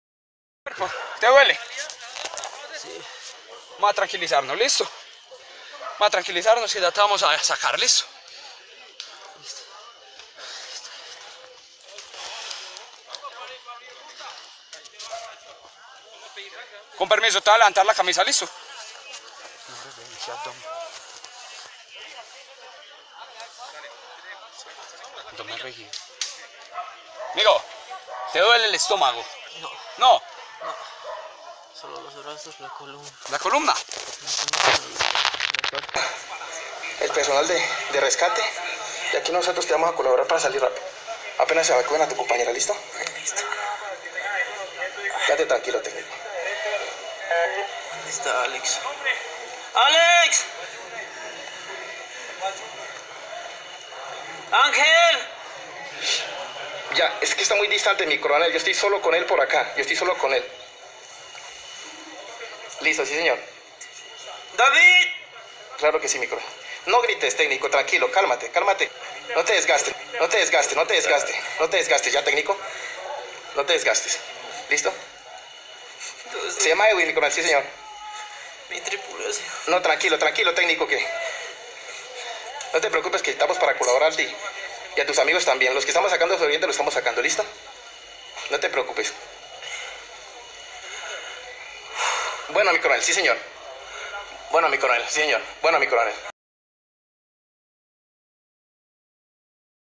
Dramático rescate de un sobreviviente en tragedia del Chapecoense
El material muestra el momento en que es encontrado el boliviano Erwin Tumiri, técnico aéreo, uno de los seis heridos que fueron socorridos tras la caída del avión BA Avro RJ85, que trasladaba al equipo brasileño de futbol.